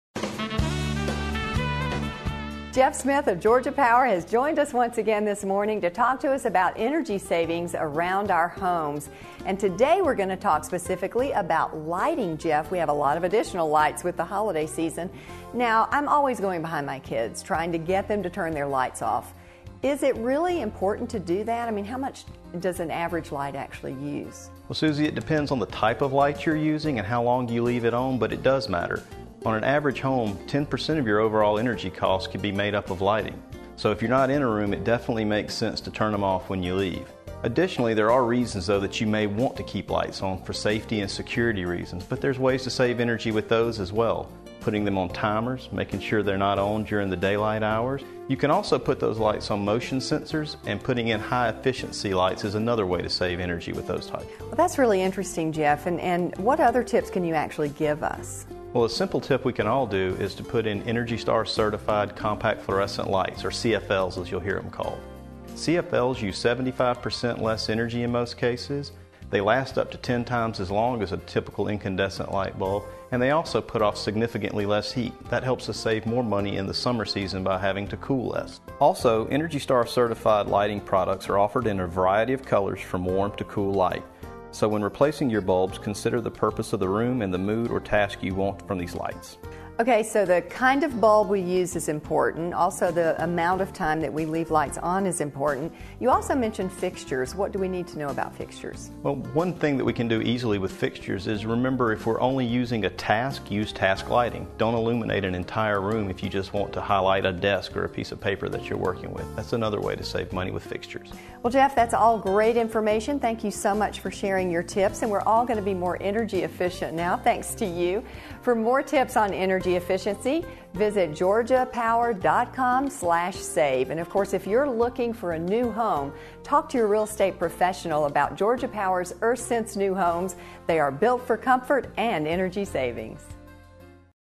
Lighting
This segment discusses tips on how to use the lighting in your home in the most efficient way.